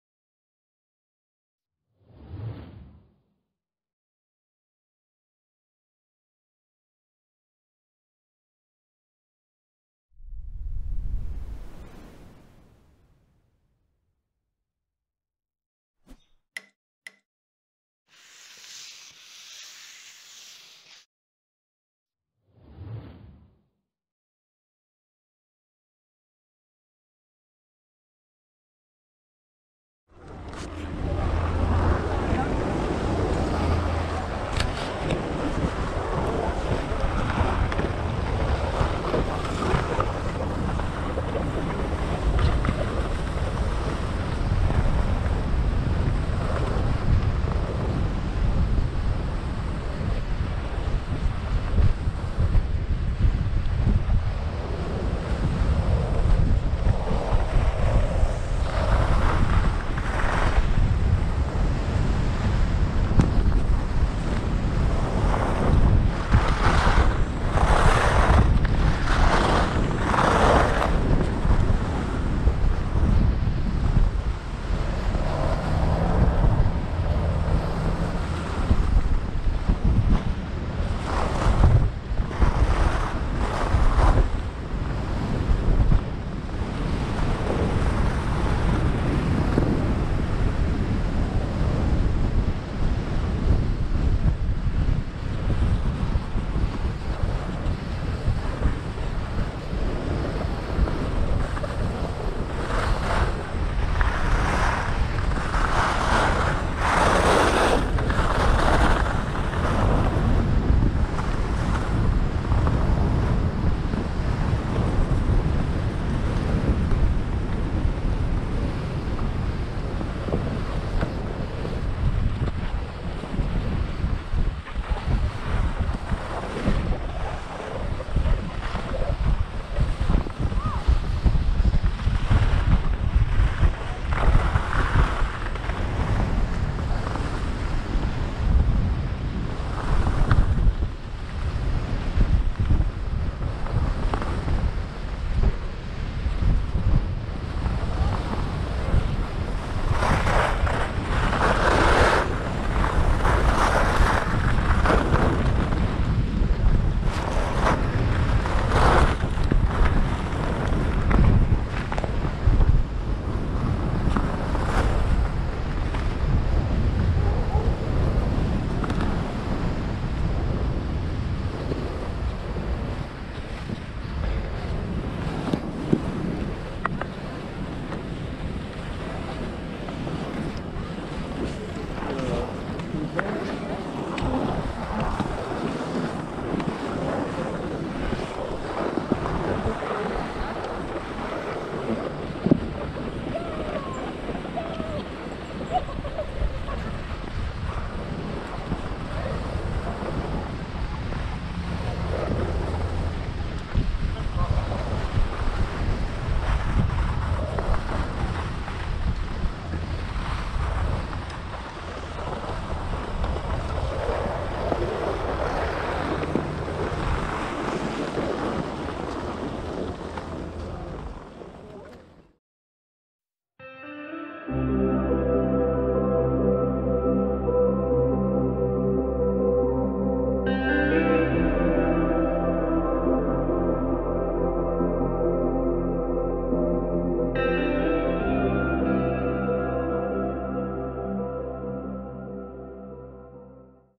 Skiing 'plan milet' nice blue piste in Tignes Val d'Isere.
About Tignes Val d'Isere ski resort:

The ski resort Tignes ​Val d'Isère is located in the Tarentaise (France, Auvergne-Rhône-Alpes, Savoie Mont Blanc, Savoie, Albertville). For skiing and snowboarding, there are 300 km of slopes and 20 km of ski routes available. 82 lifts transport the guests. The winter sports area is situated between elevations of 1,550 and 3,456 m.

Tignes and Val d'Isère are connected by lifts and slopes to form one of the largest ski resorts in France. The winter sports resort was previously called Espace Killy. It is located on the upper end of the Isère Valley. The ski resort and its slopes are partially located on glaciers. The Grande Motte Glacier in Tignes also attracts skiers in the summer.

The winter sports resort at the foot of the Pointe du Montet (3,428 m) and the Grande Motte (3,656 m) offers slopes of all difficulty levels for skiing and snowboarding. It is also considered a paradise for freeriding. The men's skiing competition for the 1992 Albertville Winter Olympics was held in Val d’Isère. The ski resort also regularly hosts World Cup races.

Filmed on GoPro Hero7 Black.
Feb 2020.